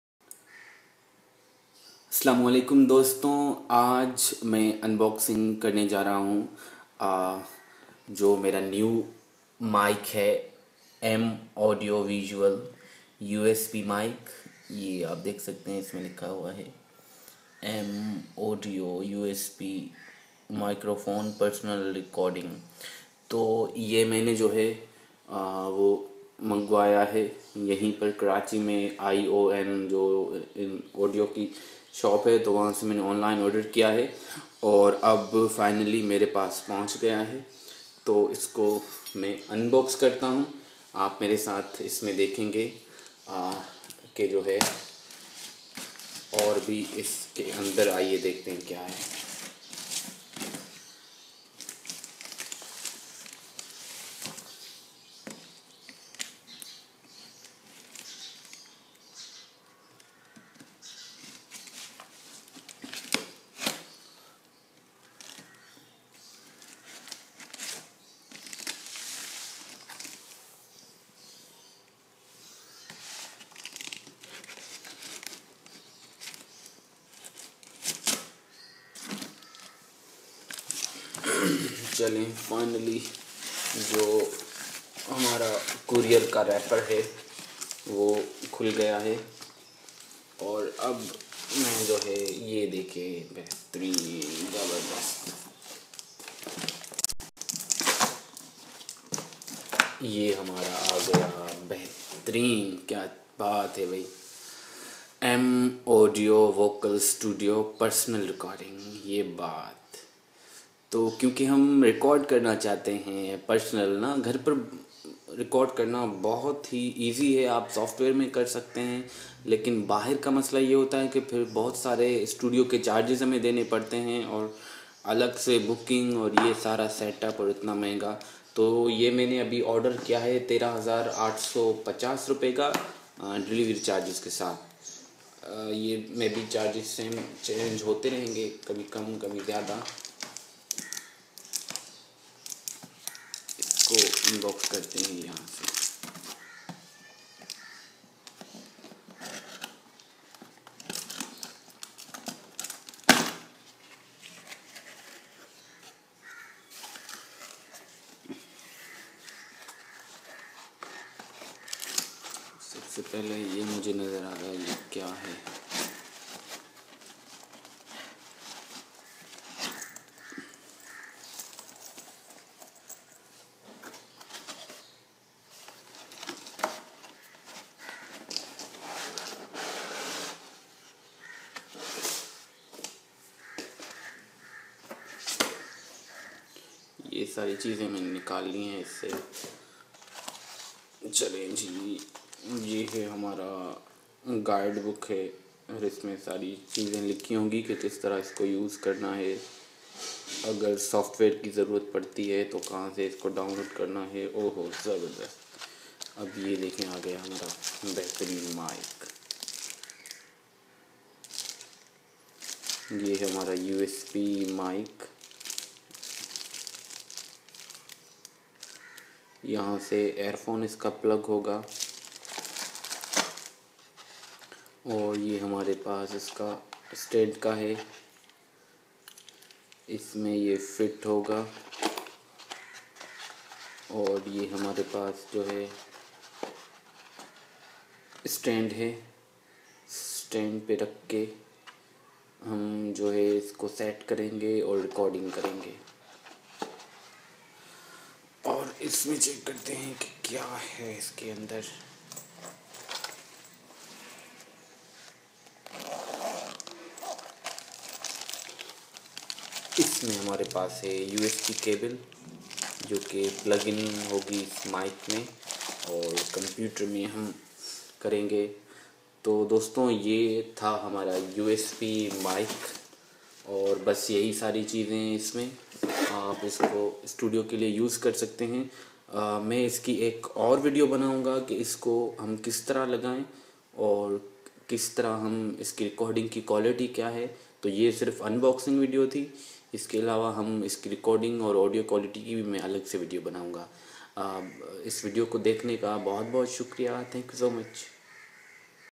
Unboxing M Audio USB Microphone in Karachi, Pakistan | Urdu/Hindi
i am unboxing my new M Audio Vocal Studio USB Mic.
Video Includes Unboxing of thi USB Mic.